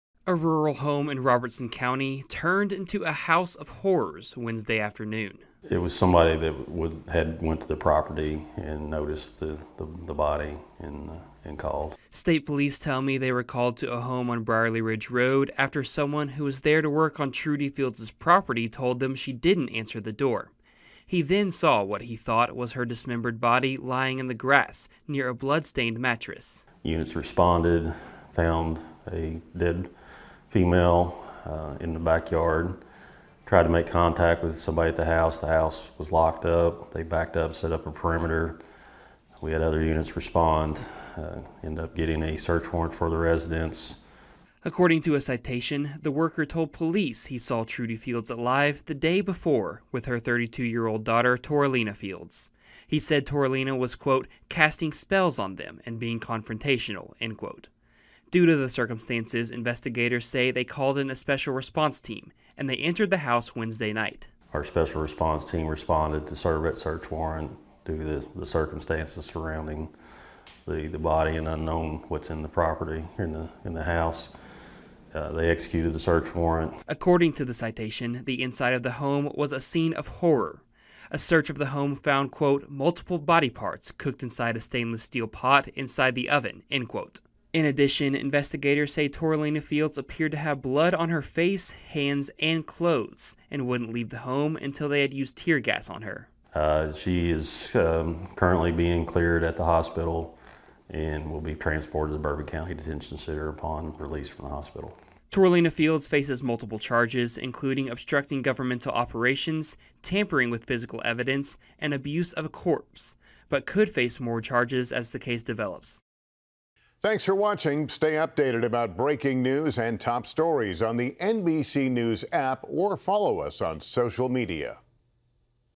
Kentucky woman found dismembered outside home, daughter arrested
A Kentucky woman was arrested after investigators found the dismembered body of her mother outside the home they shared. WLEX's Caleb Barnes has details on the arrest and investigation.